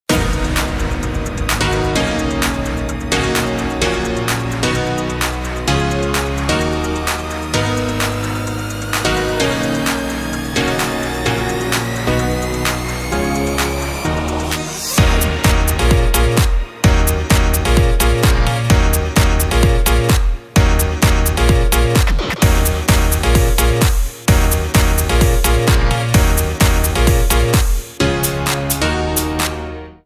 Siatkarze na wyciągnięcie ręki - kup swój karnet!
Najlepsi siatkarze na świecie, wielkie emocje oraz świetna atmosfera w nowoczesnej Hali Urania. Zajmij swoje ulubione miejsce na cały sezon, nie czekaj na moment otwarcia sprzedaż biletów. Siatkarze Indykpolu AZS Olsztyn wracają do Hali Urania - kup swój karnet już dziś!

Wejdź już teraz na bilety.indykpolazs.pl